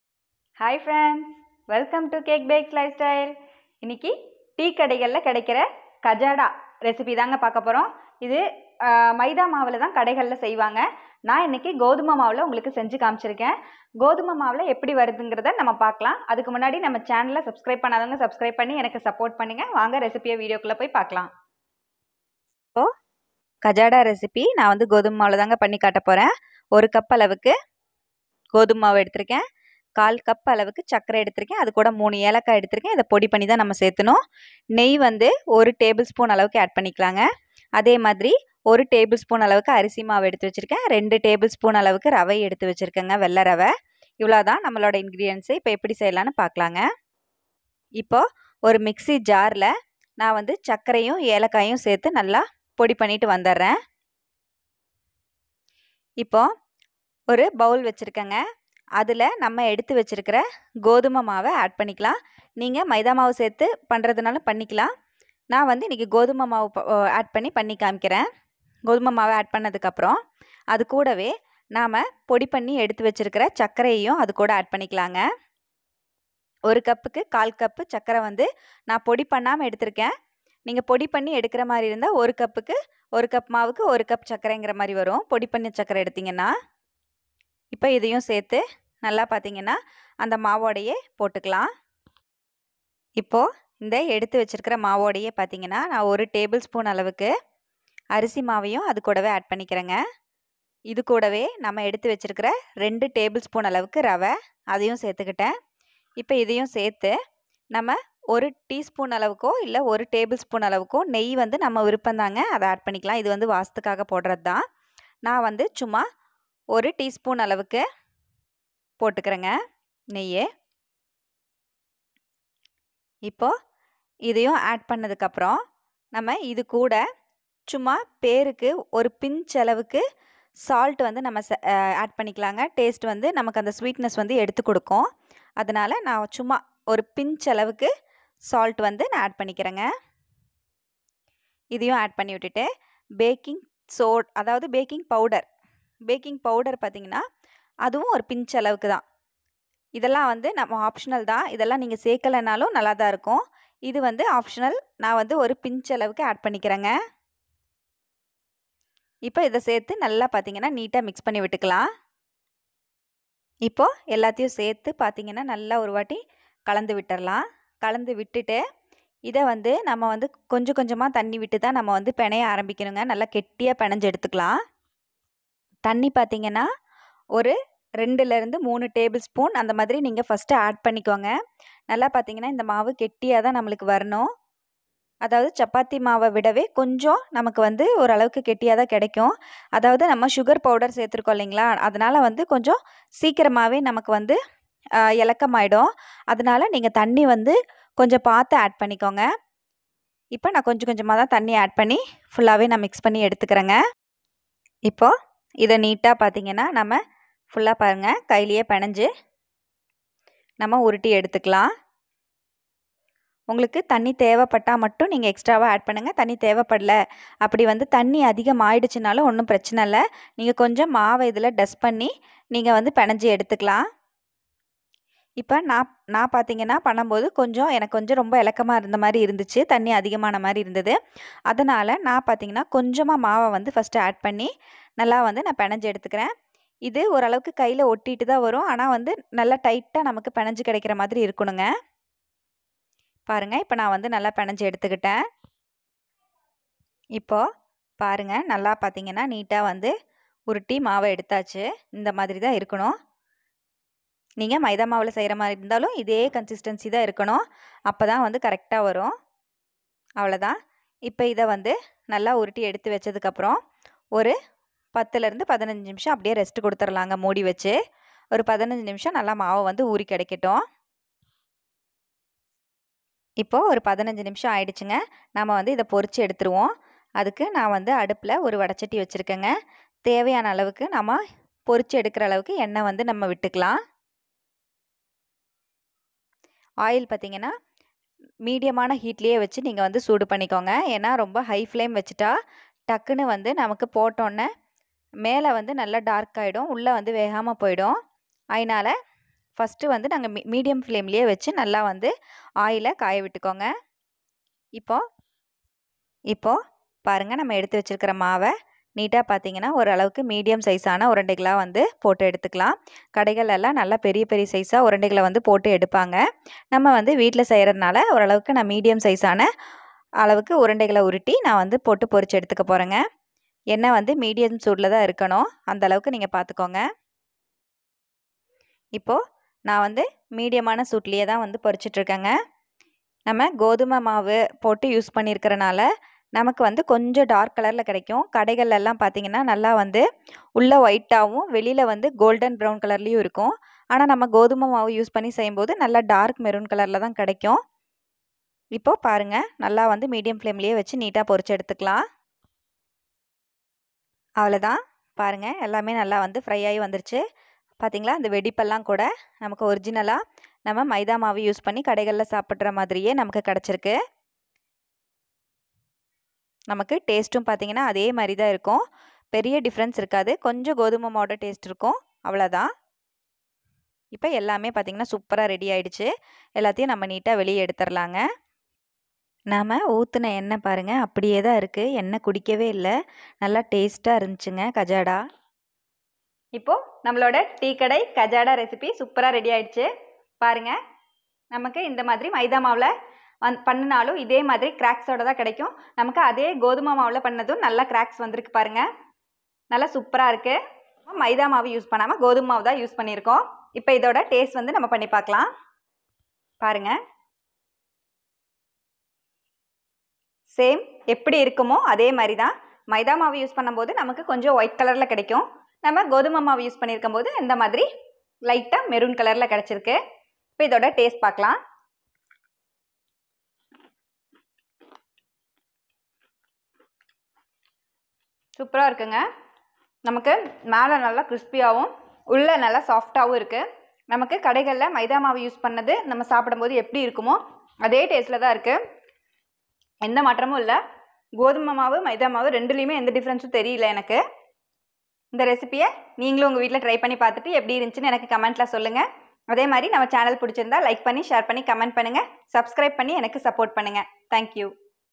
டீ கடை காஜாடா செய்முறை / Tea kadai kajada recipe/How to make kajada recipe
Kajada recipe

Rice flour 1 table spoon
Rava 2 table spoon
Baking powder 1pinch
Salt 1 pinch
Ghee 1 teaspoon ( optional)